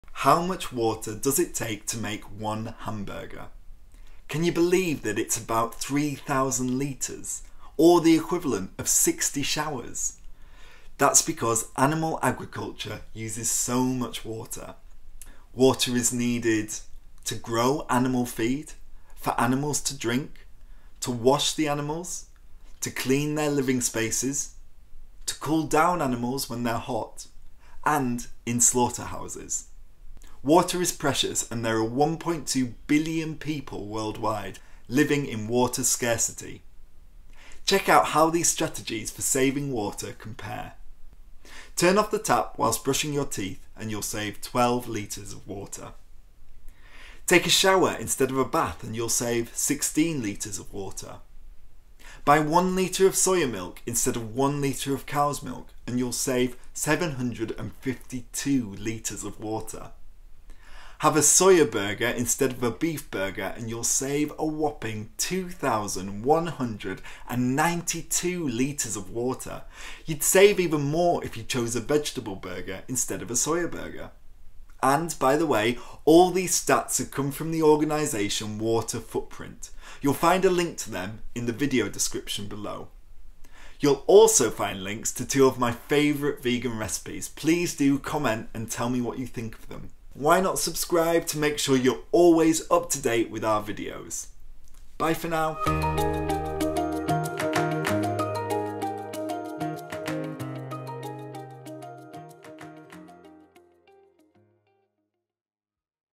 You save so much water when you switch to a vegan diet | The Veganuary Challenge
1.2 billion people worldwide live in areas of water scarcity. Water is a previous resource, and you save so much water when you switch to a vegan diet.

Today, Stuart explains why animal agriculture uses so much water, and how switching to a plant-based diet compares to other water-saving strategies.

Learn more about the sustainable use of water from Water Footprint

This is part a daily video series for January, aimed at helping you try a vegan or plant-based diet.

Receive daily emails supporting you to try a vegan or plant-based diet for one month.